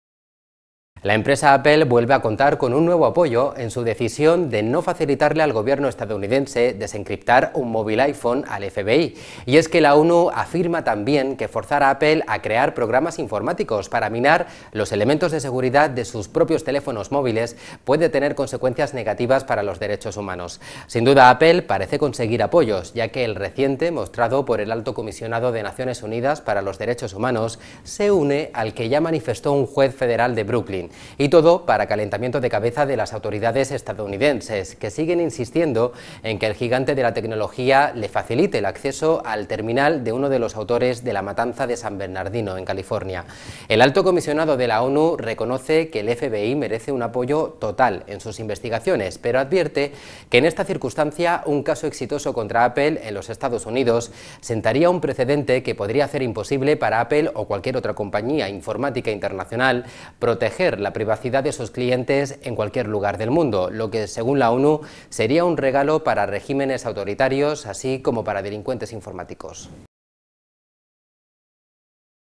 La ONU apoya a Apple en su rechazo a desencriptar un iPhone para el FBI
La empresa Apple vuelve a contar con un nuevo apoyo en su decisión de no facilitarle al gobierno estadounidense desencriptar un movil Iphone al FBI. Y es que la ONU afirma tambien que forzar a Apple a crear programas  informáticos para minar los elementos de seguridad de sus propios teléfonos" puede tener "consecuencias negativas para los derechos humanos".

English:
The Apple company retold with new support in its decision not to provide the US government to decrypt a mobile Iphone FBI. And the UN says also that forcing Apple to create software to undermine the security features of their own phones "may have" negative consequences for human rights. "